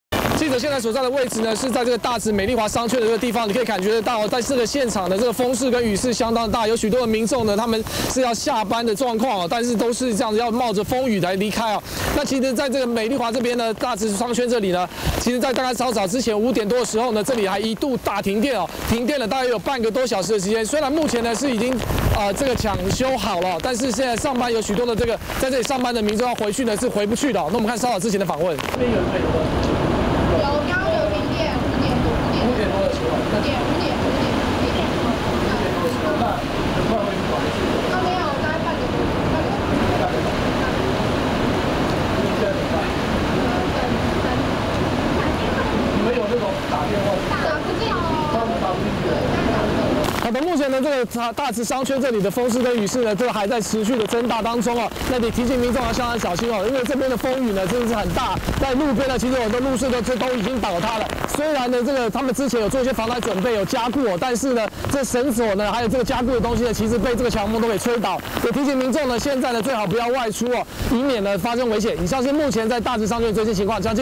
20150928中天新聞 颱風驟雨強風 北市大直大停電
颱風杜鵑來襲，台北市的大直因為強風驟雨的關係，傍晚五點左右大停電，經過半小時搶修，目前恢復供電，不過因為風強雨驟不少路樹路燈都被吹倒。